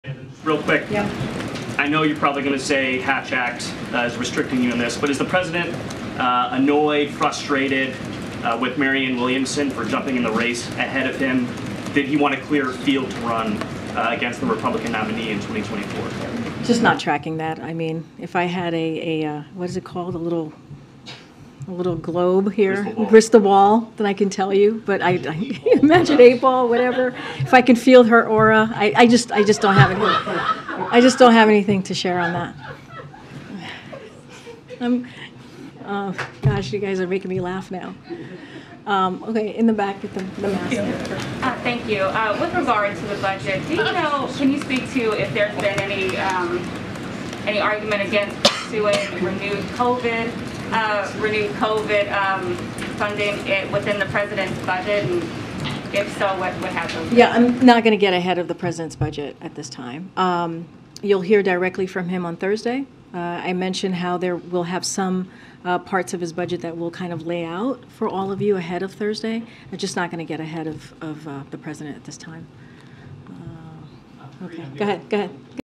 Karine Jean-Pierre Reacts To Marianne Williamson Running Against Biden
Karine Jean-Pierre is asked about Marianne Williamson entering the race for the 2024 Democratic presidential nomination.